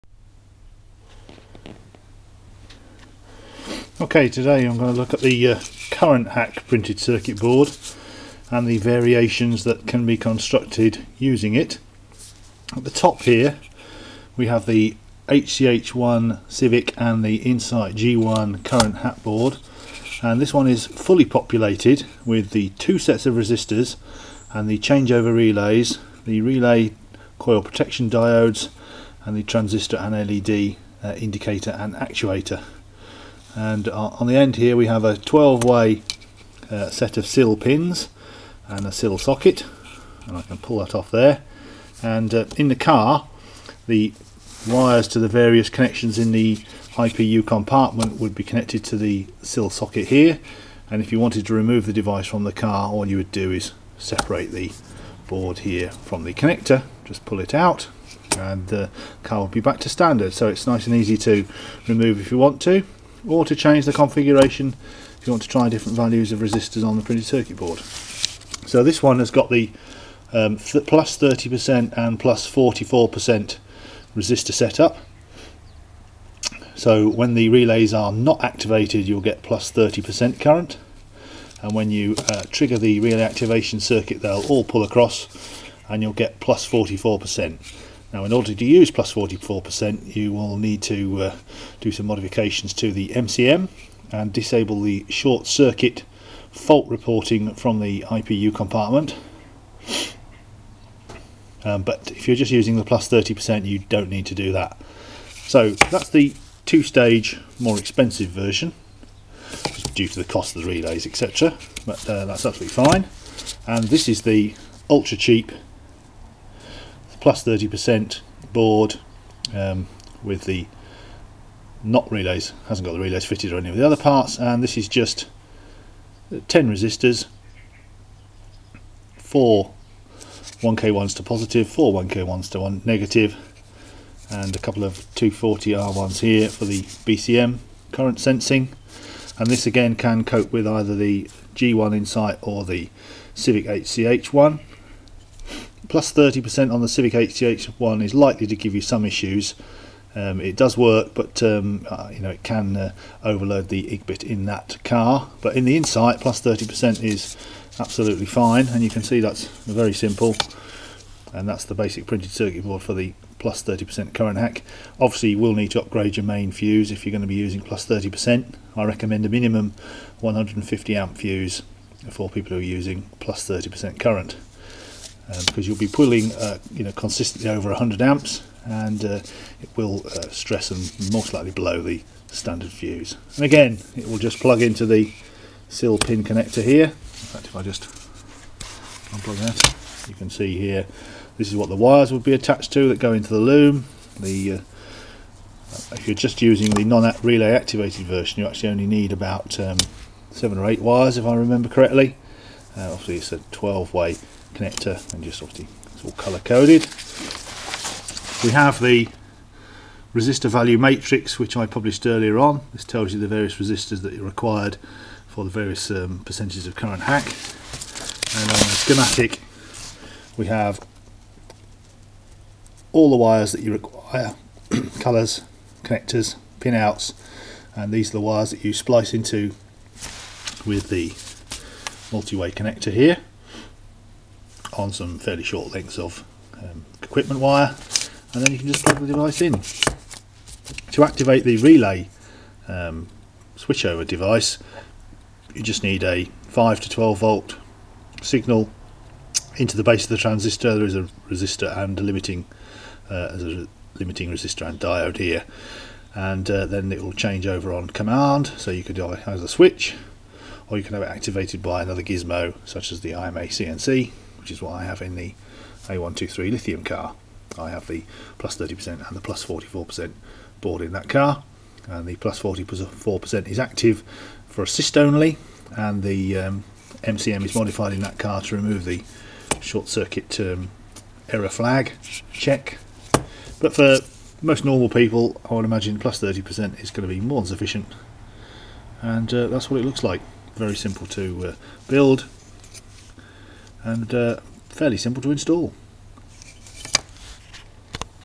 G1 Insight and HCH1 Civic Current hack variations.
Two stage more expensive relay switched +30 & +44% or single stage cheap and cheerful +30%.

Device plumbs into the car using short lengths or wire spliced into the loom and connected to a 12 way sil 0.1" female socket strip.

Unplugging the device returns the car to standard, so easy to revert if you don't like it or get problems.

Using different resistor combinations as per my posted chart a while back you can select hack values of between +10-50%. Results not guaranteed at over 30% and be prepared for issues.

Don't forget a minimum 150A fuse for hacks upto +30% and preferably a 175/200A one above +30%.